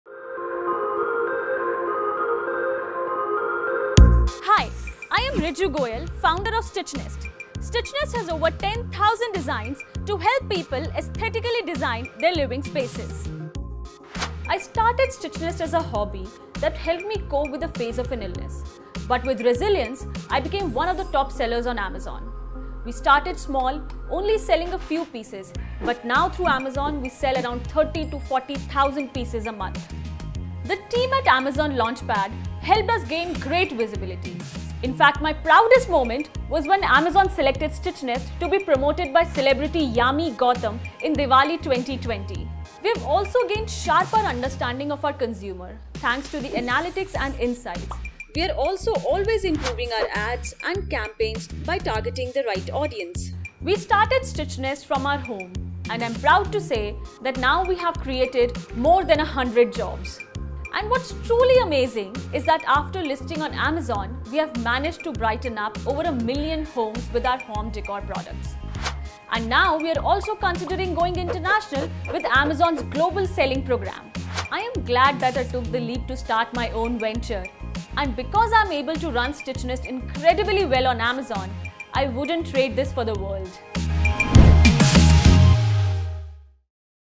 Amazon x TOI Spotlight | Riju Goyal | Aspire To Inspire - Stitchnest
Riju Goyal started home decor-based venture Stitchnest just as a hobby, but with help from Amazon, she has been able to scale her business to greater heights. Watch Riju's success story and find out how Amazon played a key role in it.